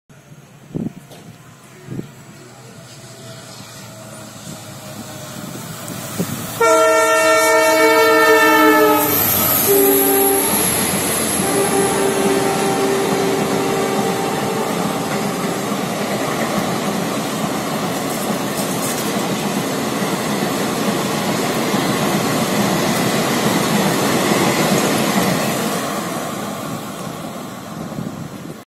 தூத்துக்குடி-மைசூர் விரைவு வண்டி||Tuticorin-Mysore Express Crossing Top Speed Near Madurai@night cap
Mysore–Thoothukudi Express is a daily train service between the cities of Mysore and Thoothukudi